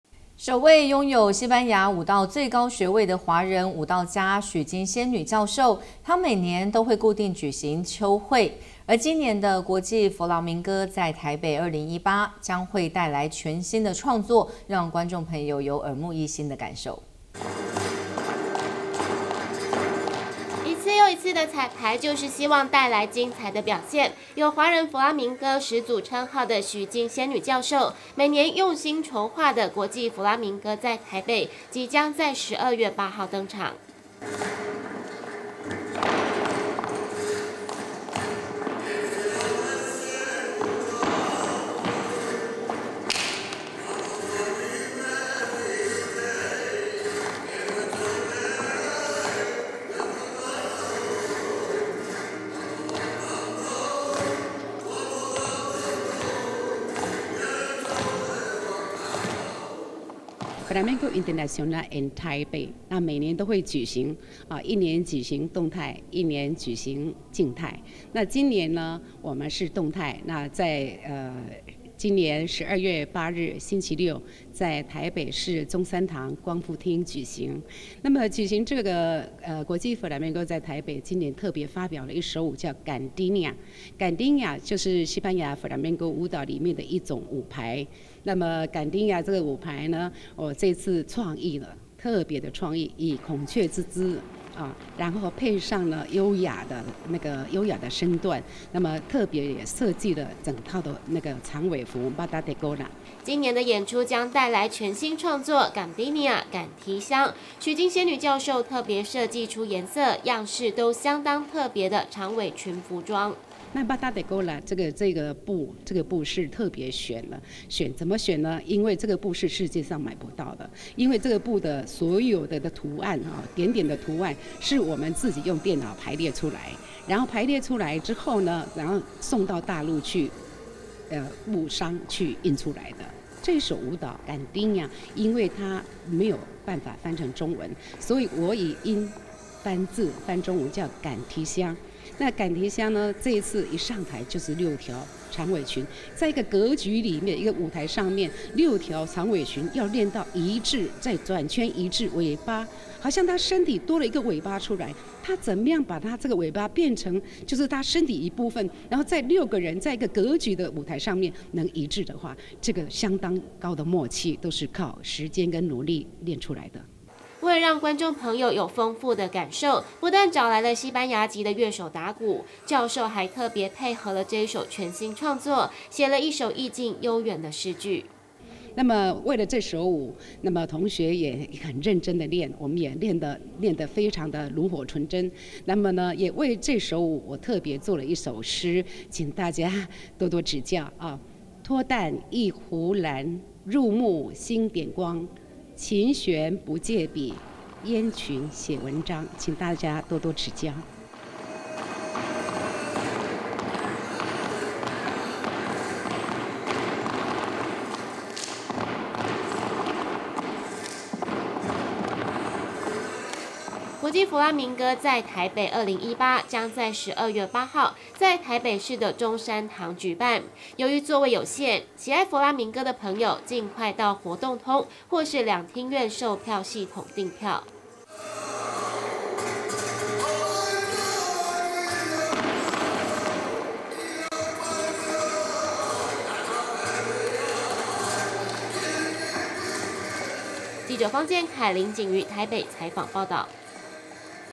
天外天新聞1071119 15佛拉明哥在台北 帶來全新創作
首位擁有西班牙舞蹈最高學位的華人舞蹈家許金仙女教授，每年都會固定舉行「秋會」，今年的「國際佛拉明哥在台北2018」，將會帶來全新的創作，讓觀眾朋友有耳目一新的感受。